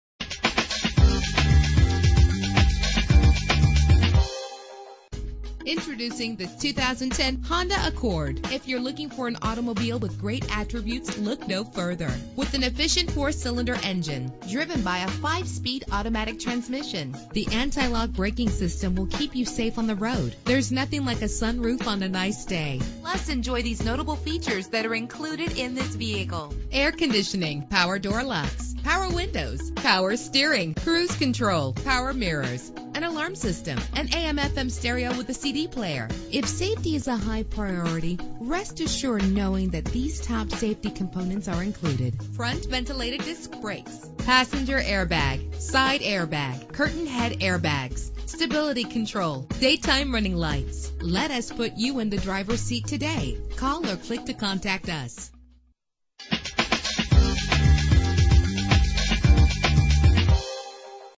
2010 Honda Accord - Duluth GA
Year: 2010
Make: Honda
Model: Accord
Trim: 2.4 EX
Engine: 2.4 liter inline 4 cylinder 16 valve
Transmission: 5-Speed Automatic
Color: Mystic Green Metallic
Mileage: 25447
Address:
3325 Satellite Blvd